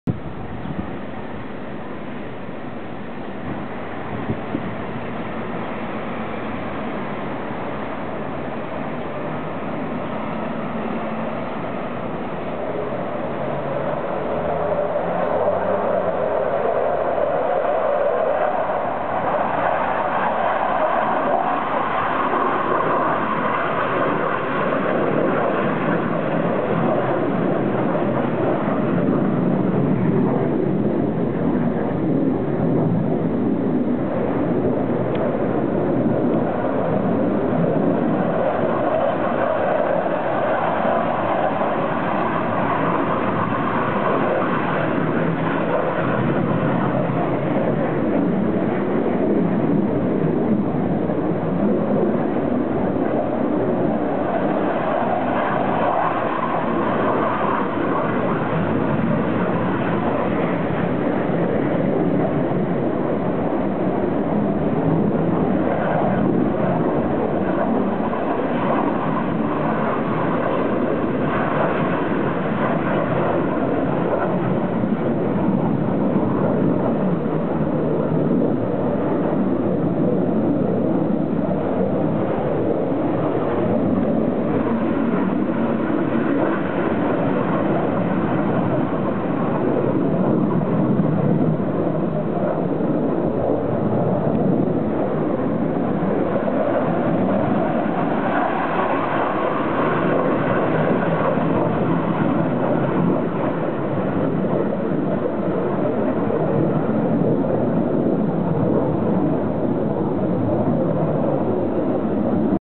F15's at Kadena, Okinawa
The island of Okinawa has a large US military presence. Naturally they like to play with all those expensive big boys' toys. Here are six F15's taking off from Kadena airbase. Sorry about the last two but they caught me out a bit. Usually they take off in twos and fours.  Six on the trot is slightly unusual.